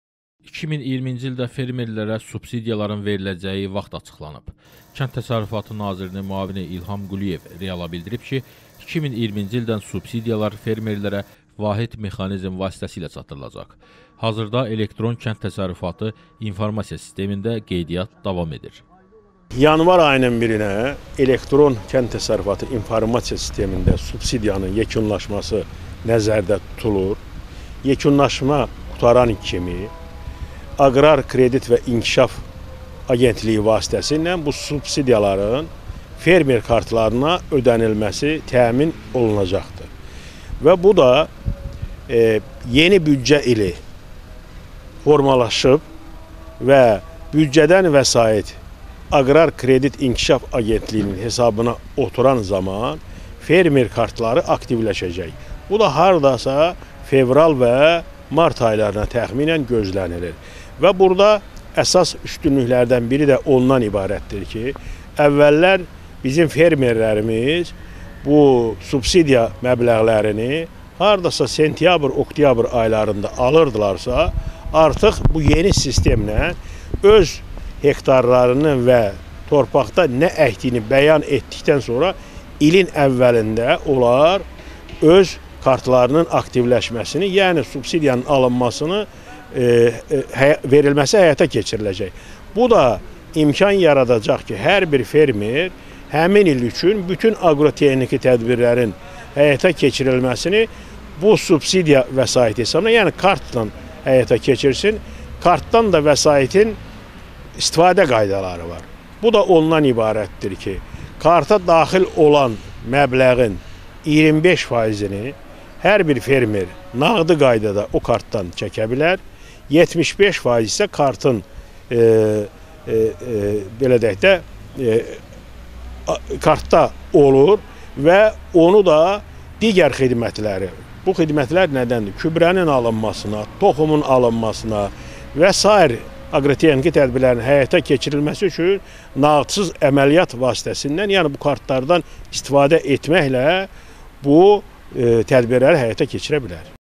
Yeni qaydalarla subsidiya
2020-ci ildə fermerlərə subsidiyaların veriləcəyi vaxt açıqlandı